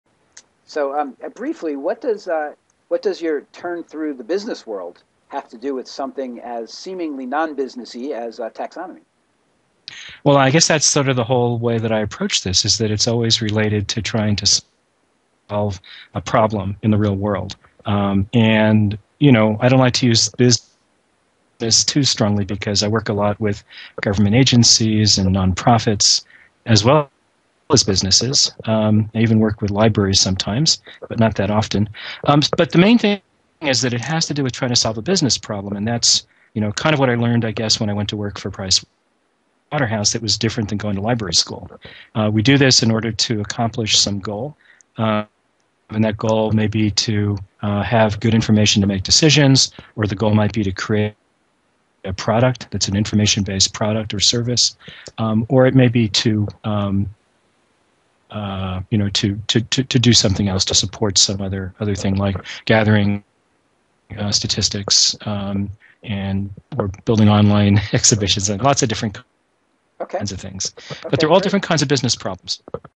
Joseph Busch Business and IA
This video is part of a course on Information Architecture taught by Bob Boiko at the University of Washington.